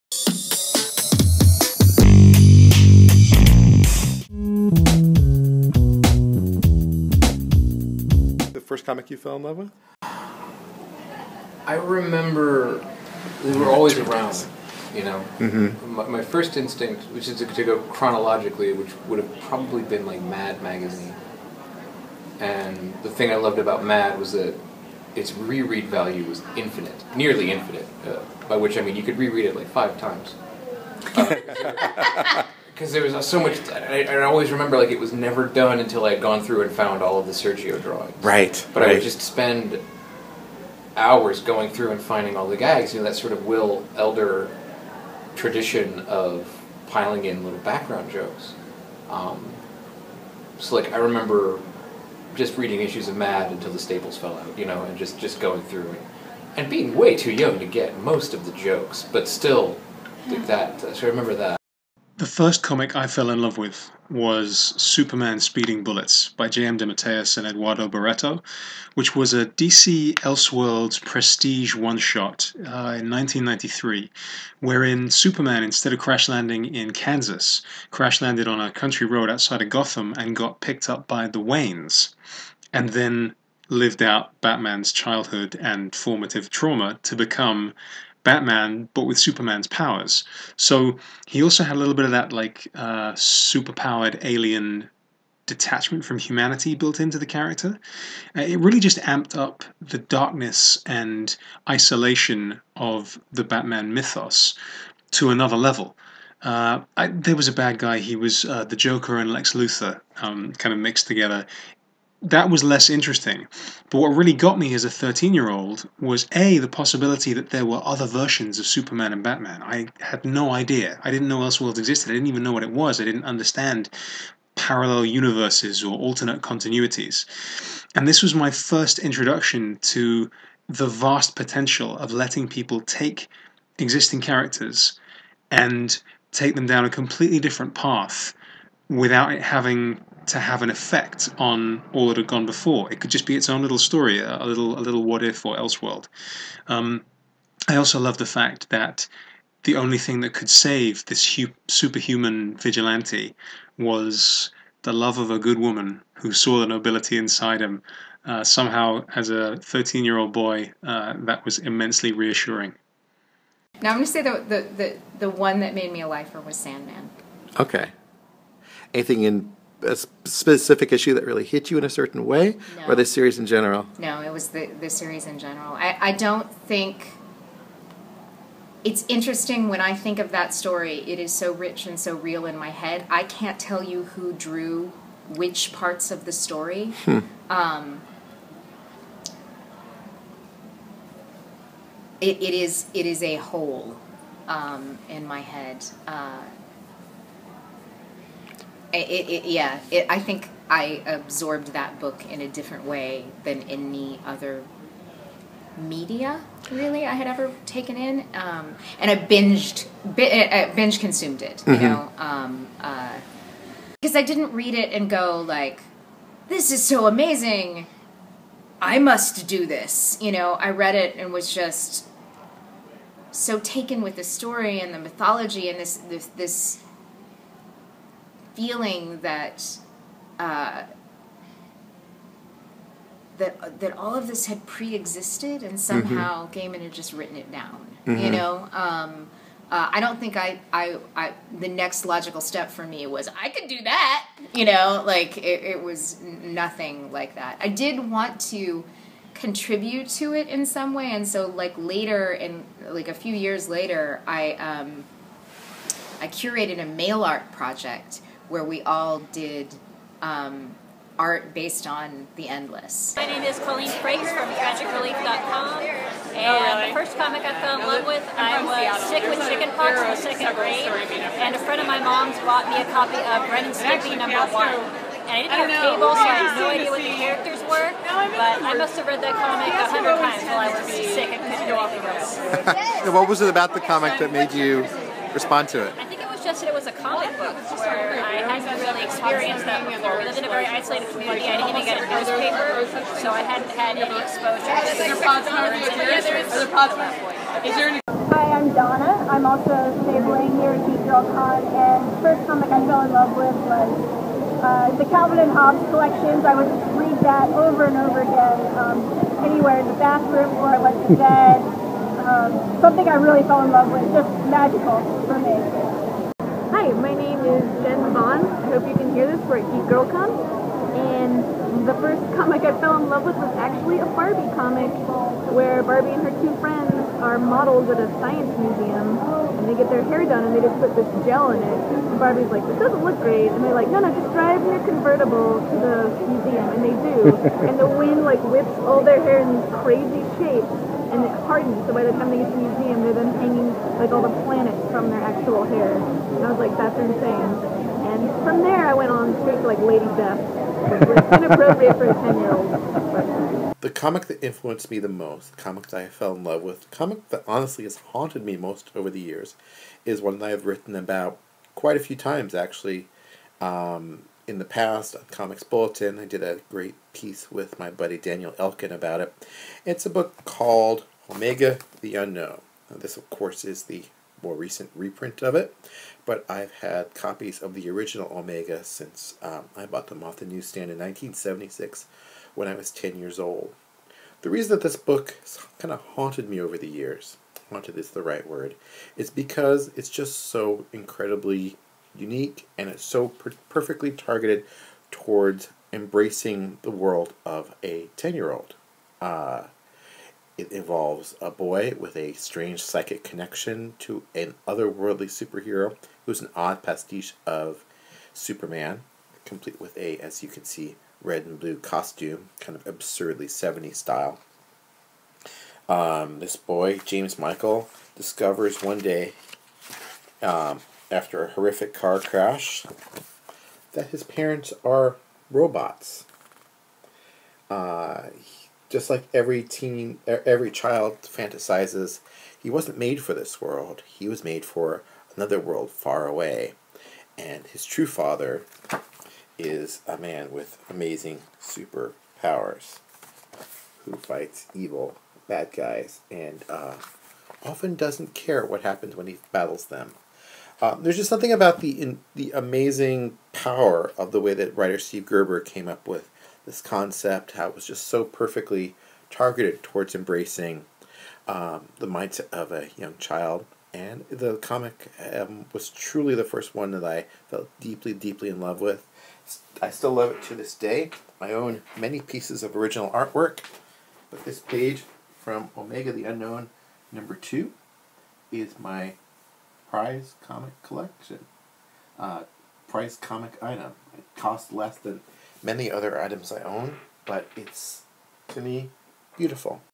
Serious Issues Ep. 4 - The First Comic You Fell In Love With
Featuring Matt Fraction, Kelly Sue DeConnick, Colleen Frakes, Donna Almendrala, Jen Vaughn, Taylor Lilley and Jason Sacks.